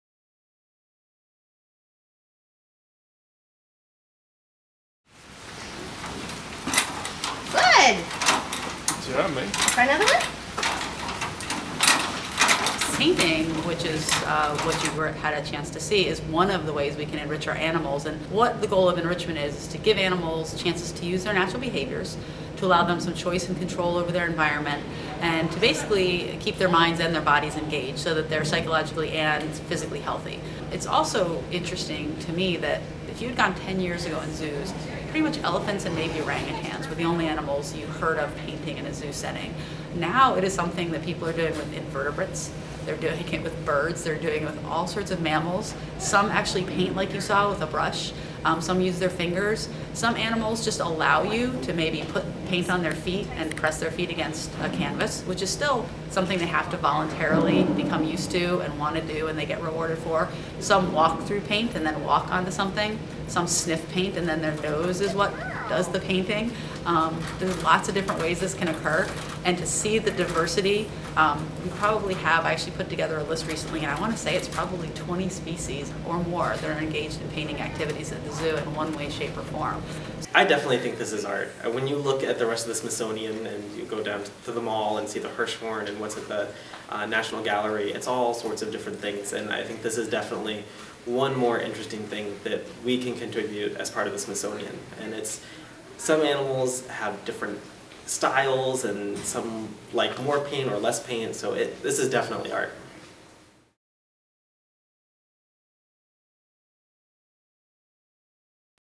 Painting Animals
Animals at the Smithsonian National Zoological Park are making paintings with their paws, tails and feet. Some hold paintbrushes, and others walk on canvases dotted with paint. To read the story and see the slide show by SHFWire reporter Jessica Sabbah, go